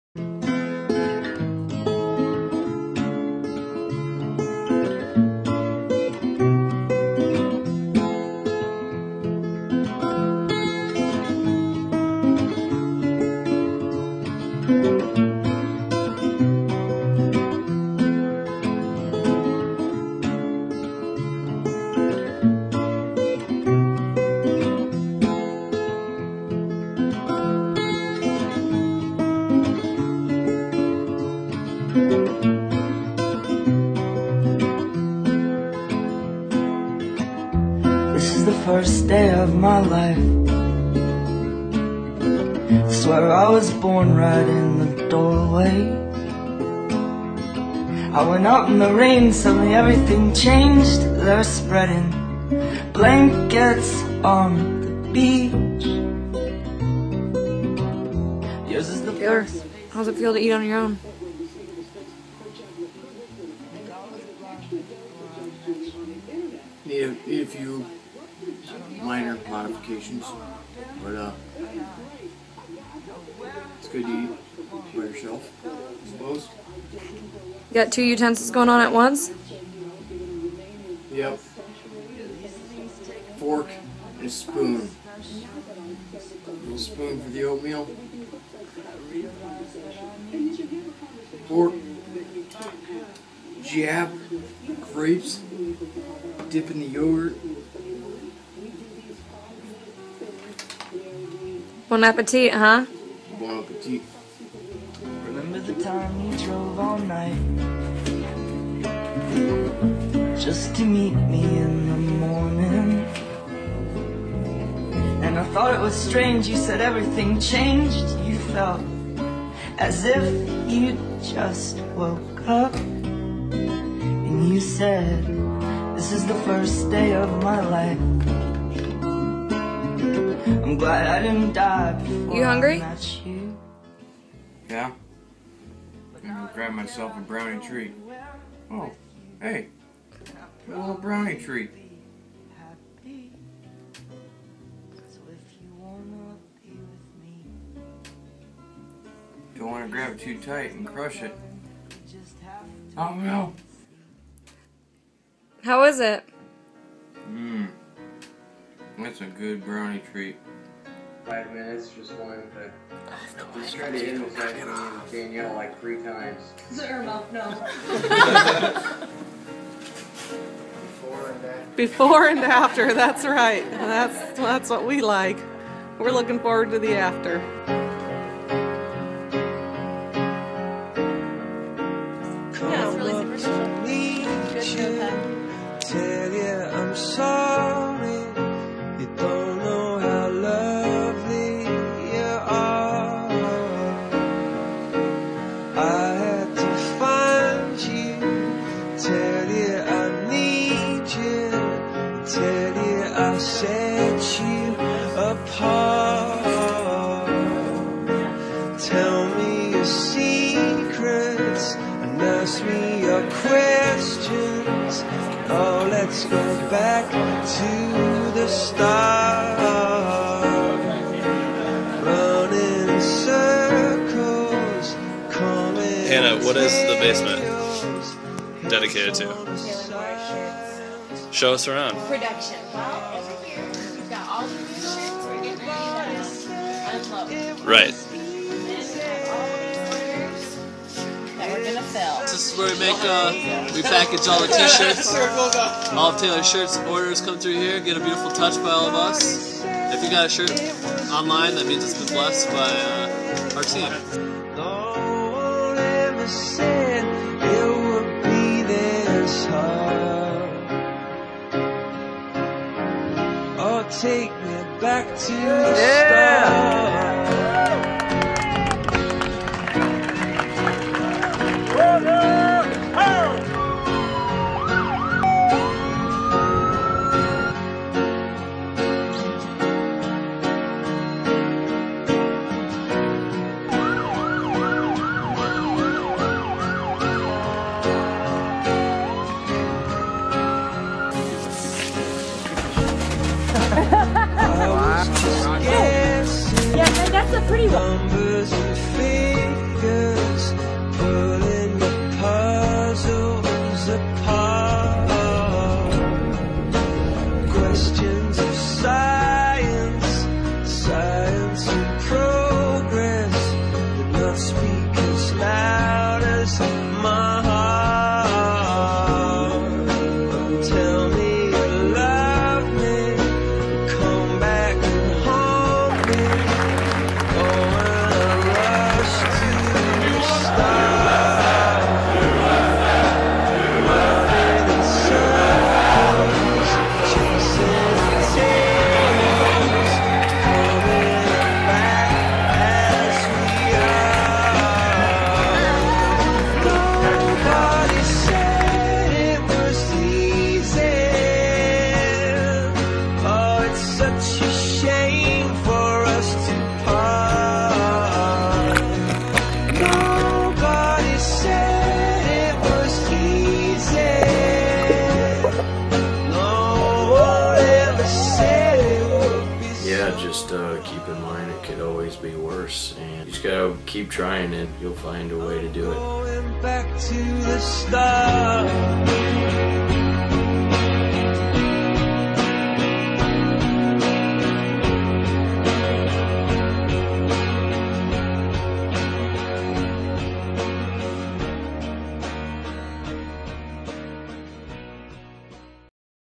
Taylor Morris, an injured veteran, and their Love Story that will take your breath away
Read more about this honorable and strong man, Taylor Morris and his girlfriend, Danielle Kelly here: